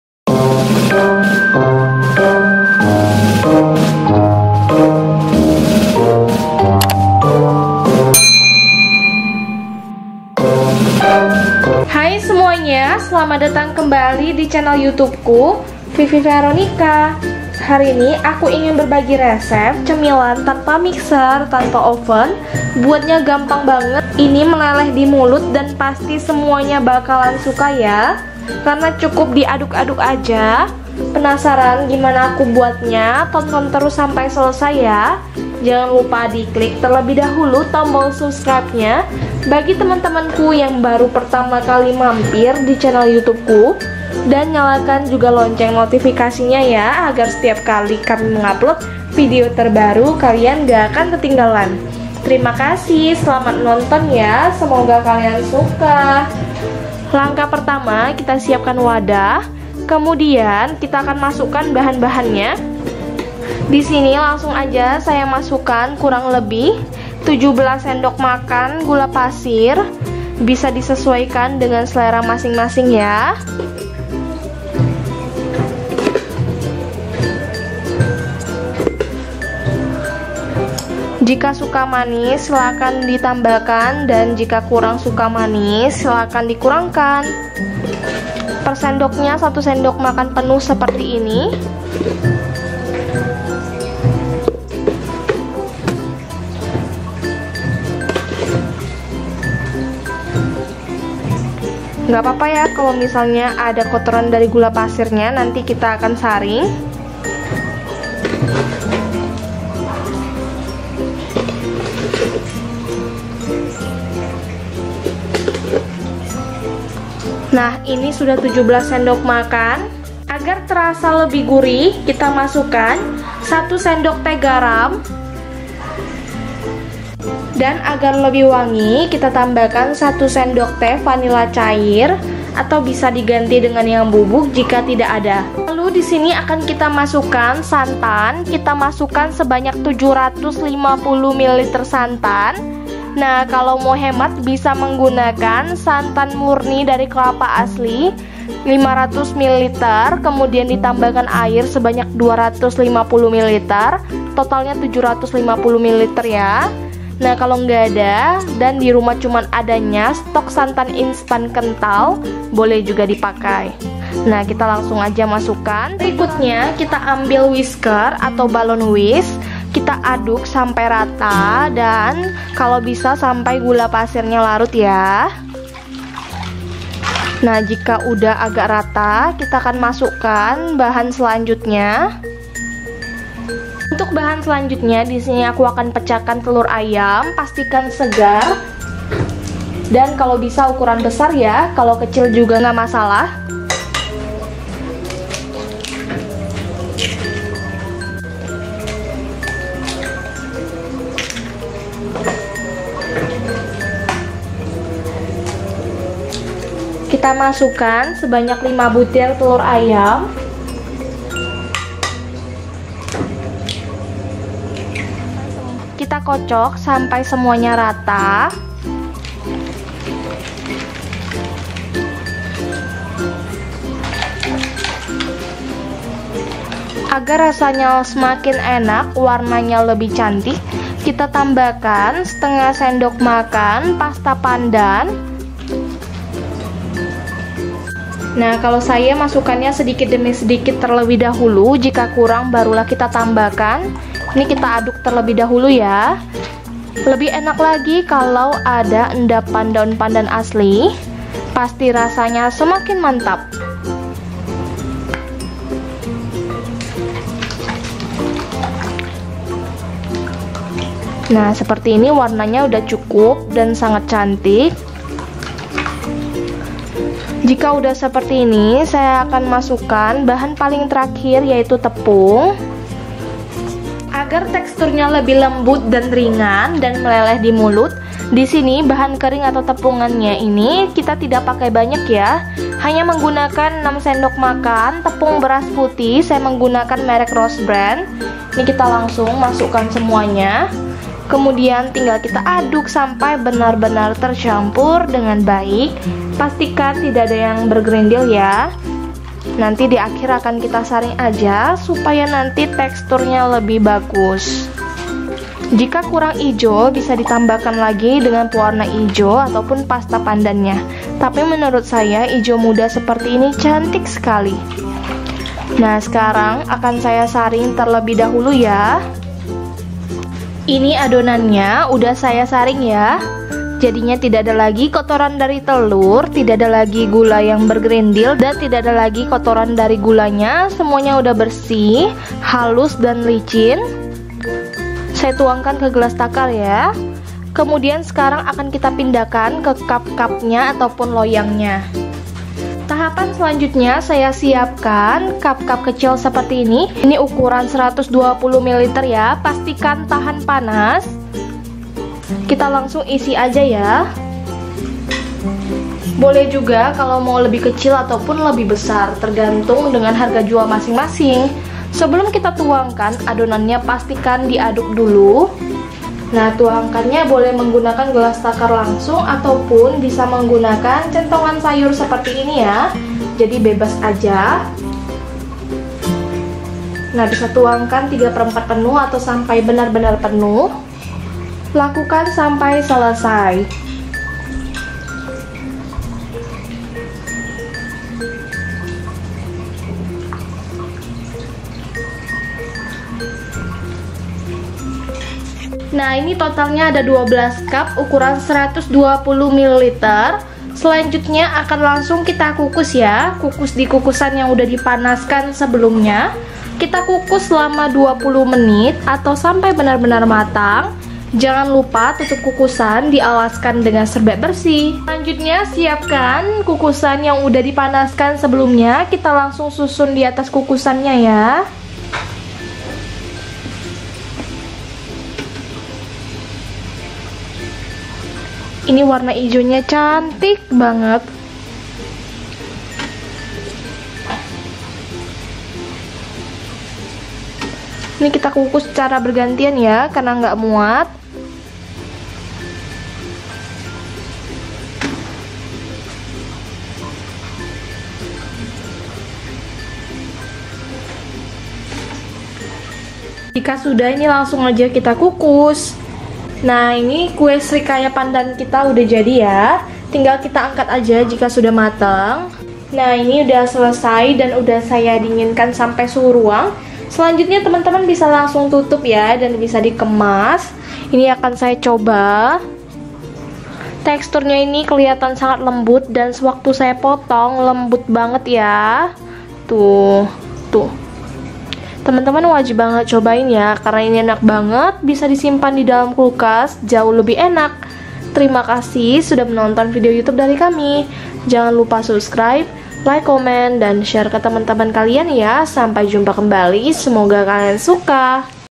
Woow...KUE INI SANGAT CANTIK CUMA DI ADUK DAN DI KUKUS RASANYA ENAK BANGET!!
Bahan :

17 sdm gula
garam
vanila
500 ml santan murni + 250 ml air
5 telur
pasta pandan
6 sdm tepung beras

1 resep menghasilkan 12 cup